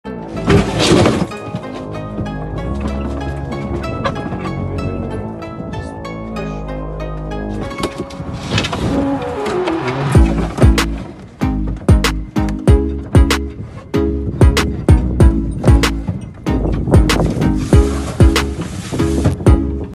Cheetah Reintroduction in India || First Look || Welcome Home ❤️
This video is courtesy of DD News. It shows footage of cheetah transferred from one cell to another.